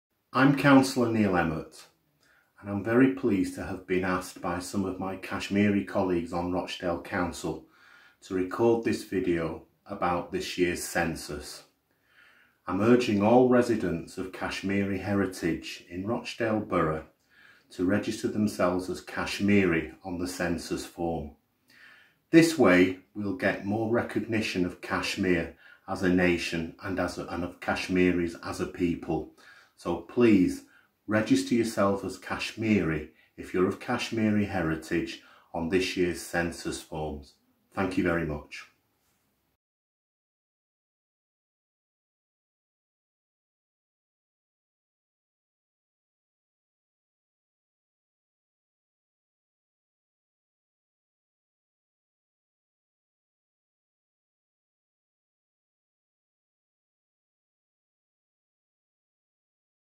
Cllr Neil Emmott Rochdale Borough Council urges Kashmiris to fill out Census forms correctly
British Kashmiris - Be Visible, Get Recognised!
It's not too late to fill in Census 2021 form.
In ethnic origin and national identity sections, tick other and write in "Kashmiri"